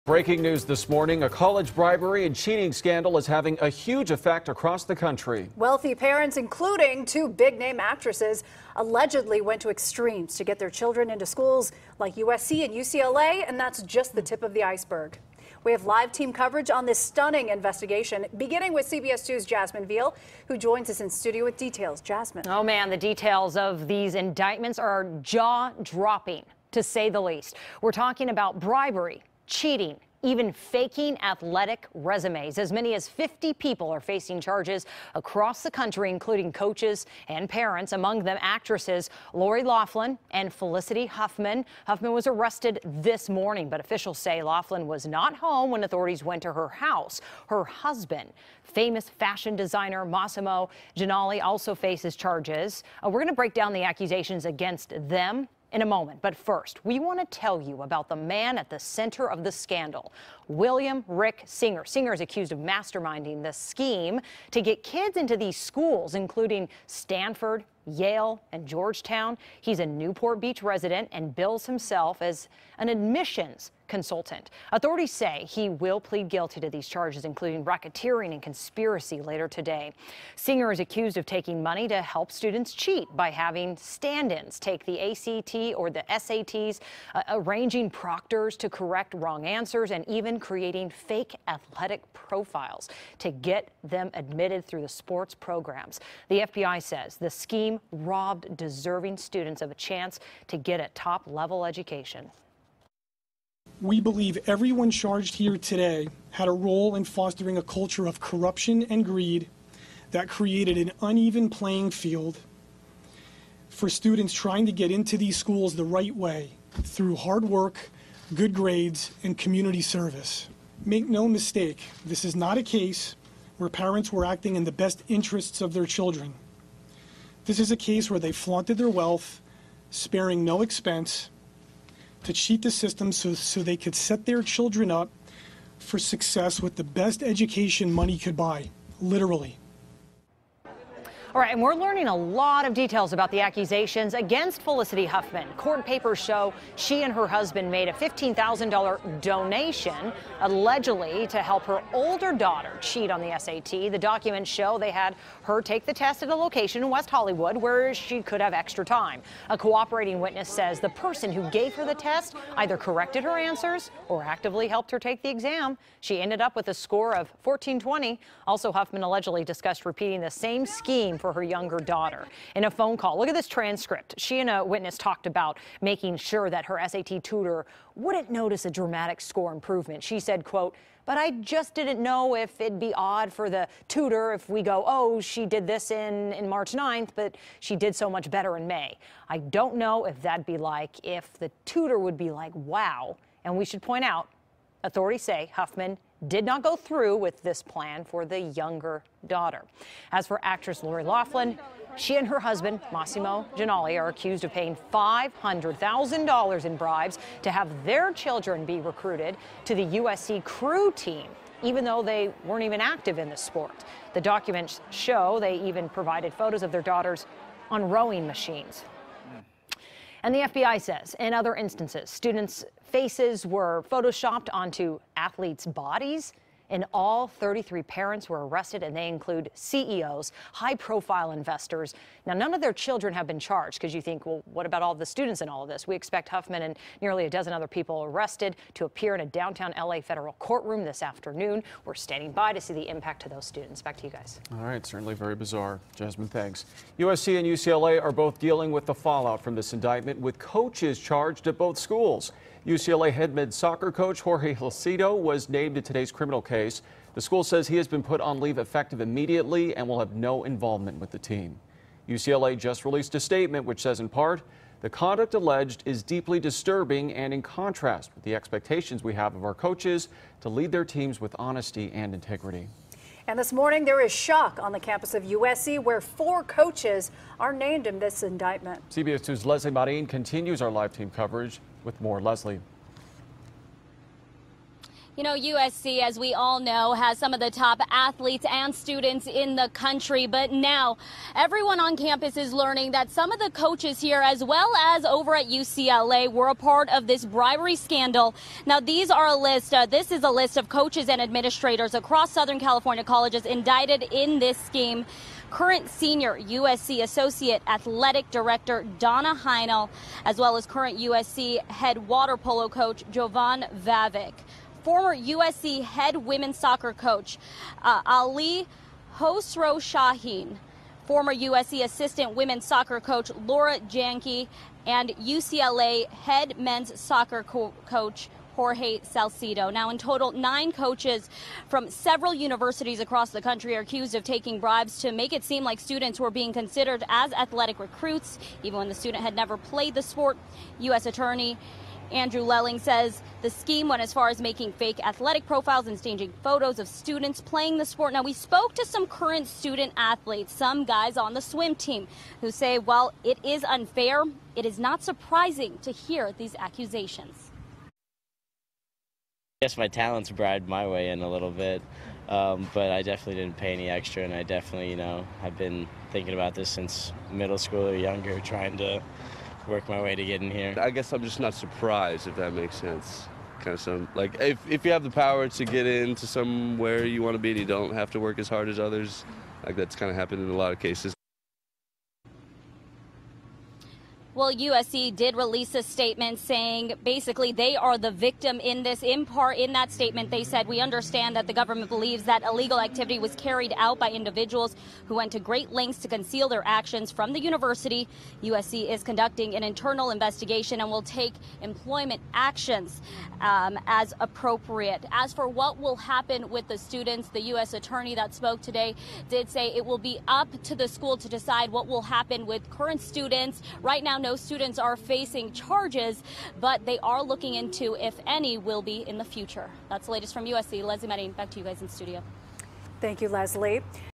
USC, UCLA Named In Shocking College Admissions Bribery Scandal; Lori Loughlin, Felicity Huffman Amon
Actresses Lori Loughlin and Felicity Huffman are among approximately 50 people indicted Tuesday in a widespread and sophisticated college admission bribery scheme in which parents are accused of paying off college coaches and standardized testing administrators millions of dollars in order to get their children into elite universities.